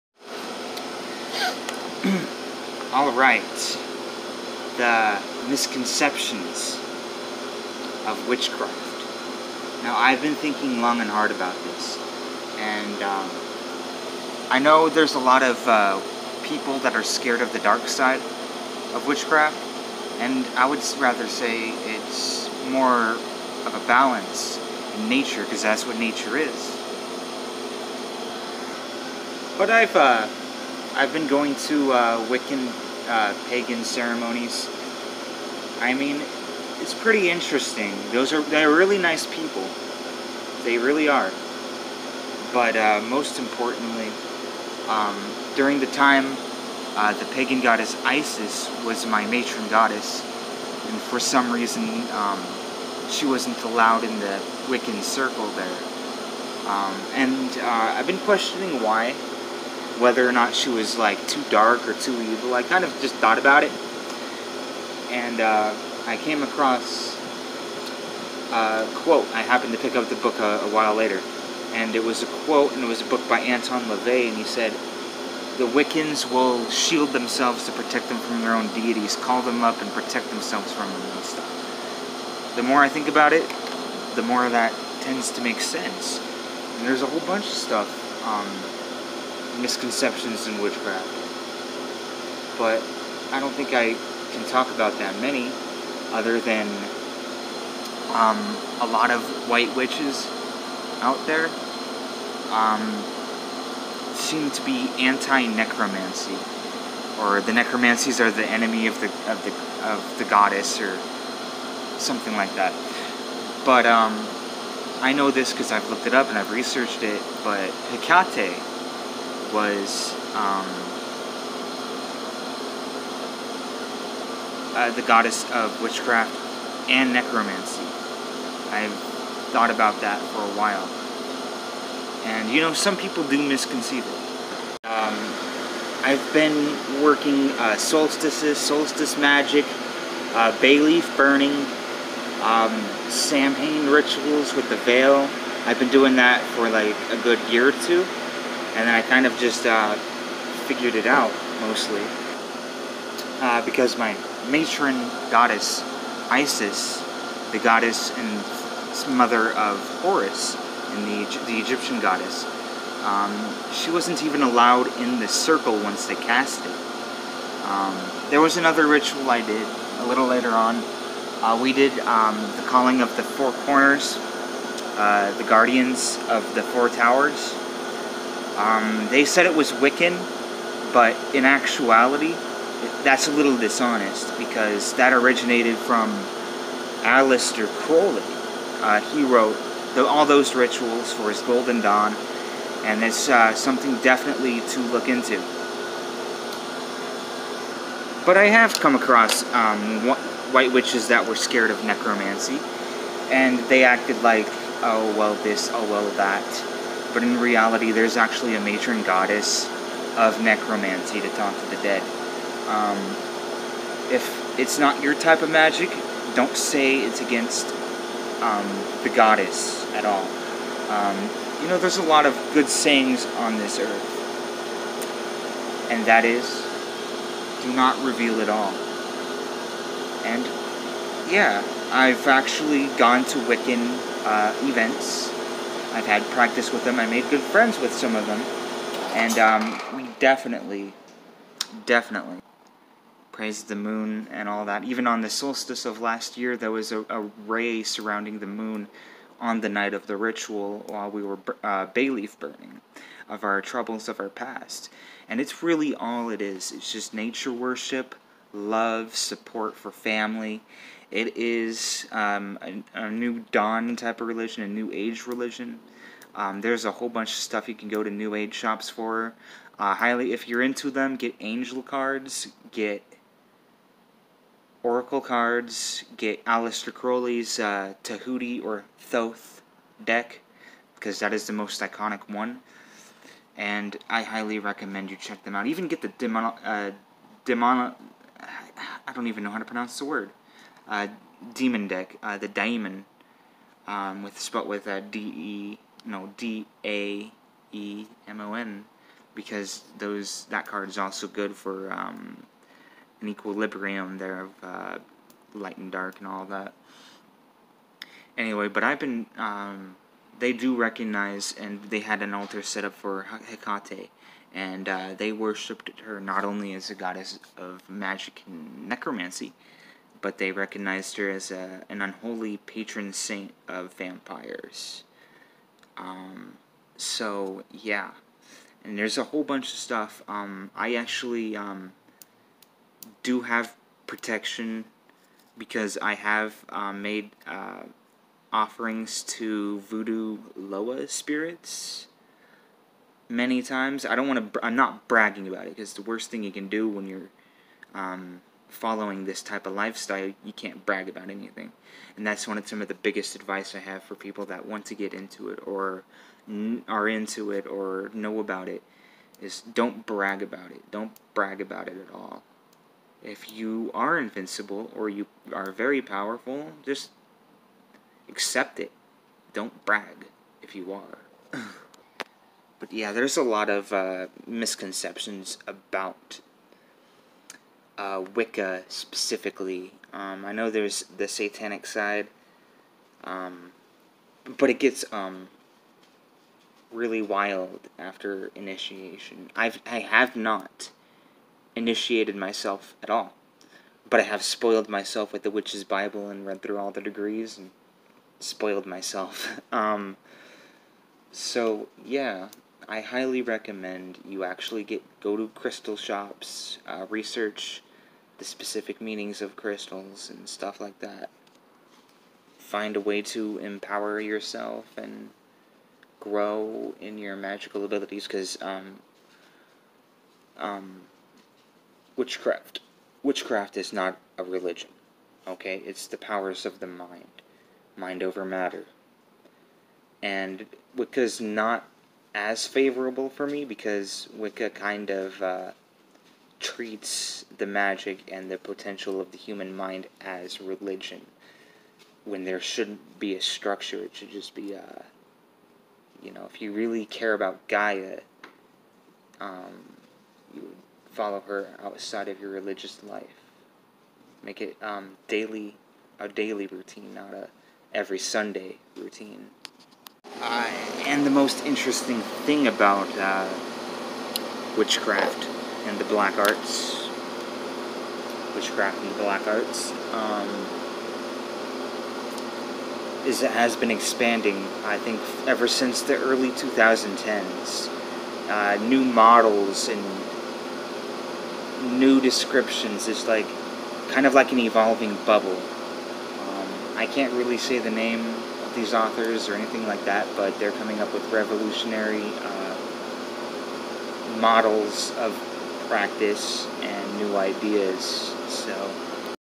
Misconceptions of Wicca
Truth is everything in Wicca is rewritten from Crowley